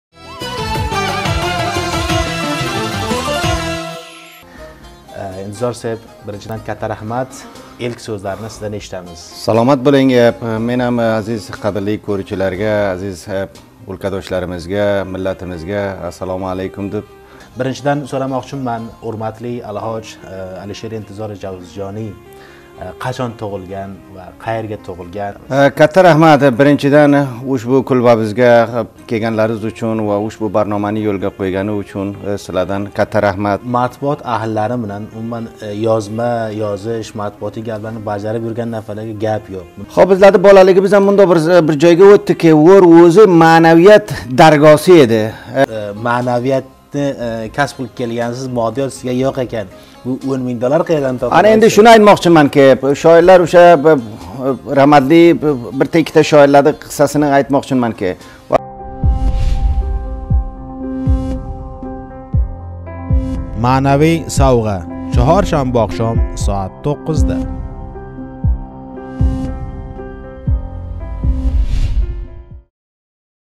معنوی ساوغه، اقتصاد اۉقیگن ادبیات‌چی، چهارشنبه(حوت۷)، ساعت ۰۹:۰۰ده.
bek tv